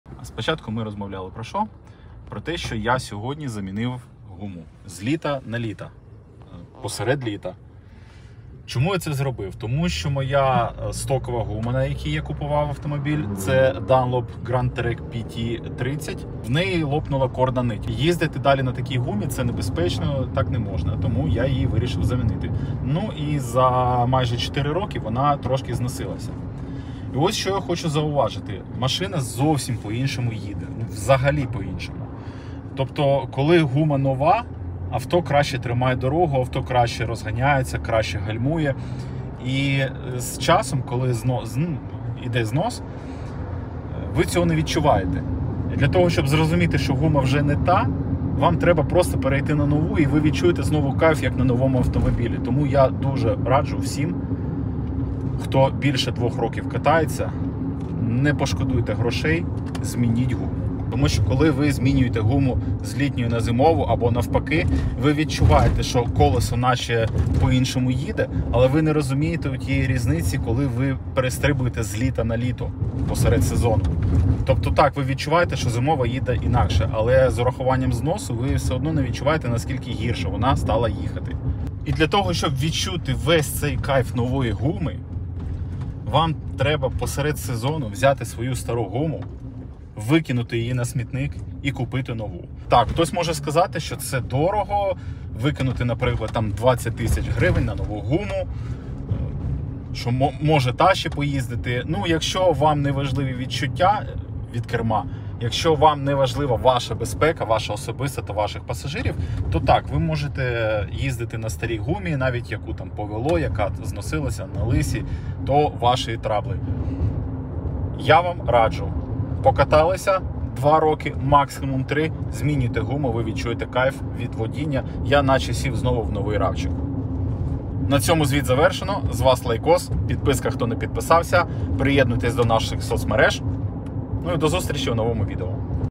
Чому НЕ варто економити на гумі.
Гроші на вітер? Змінив літню гуму в середині сезону.